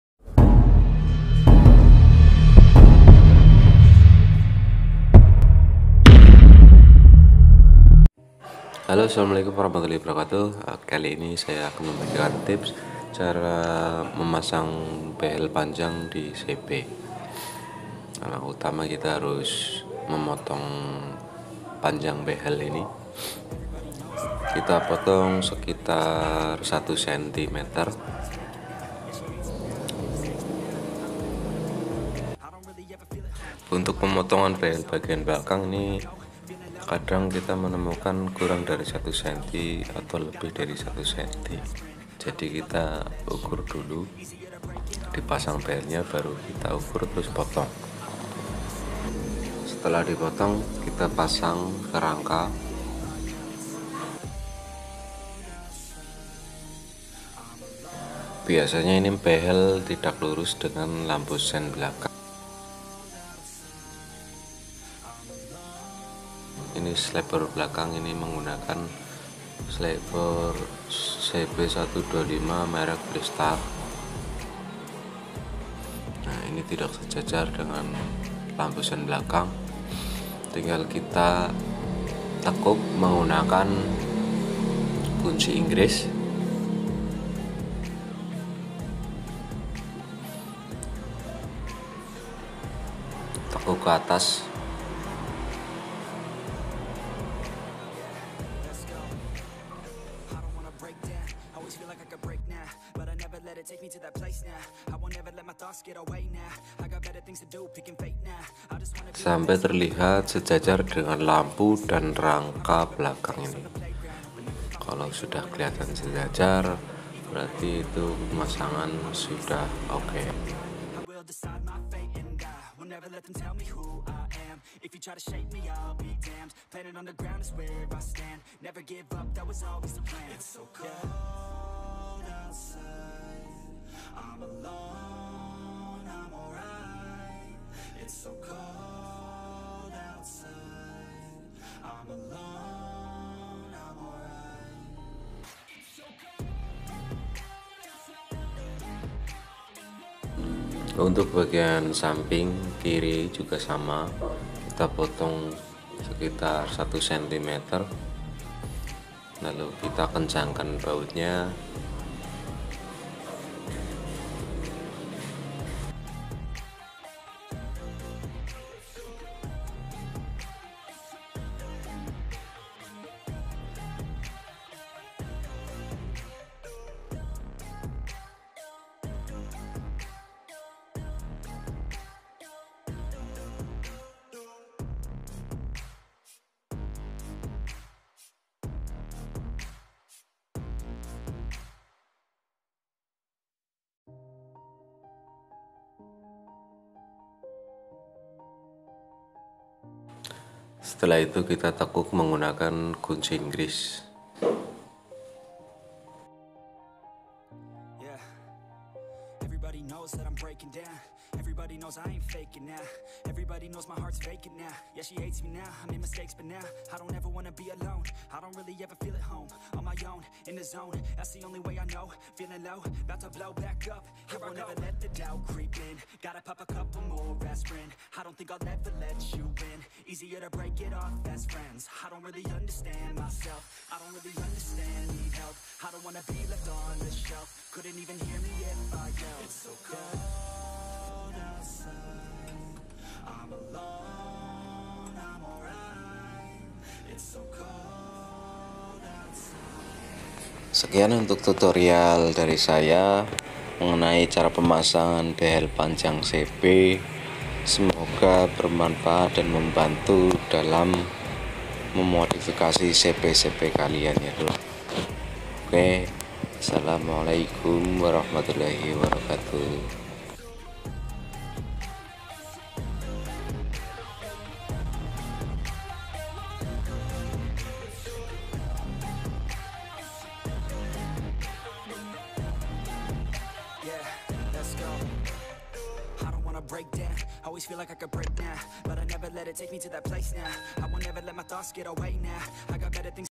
memasang behel panjang di CB tatanan
berikut cara memasang behel panjang di CB yang rangkanya sudah tatanan, simak videonya sampai habis ya genk..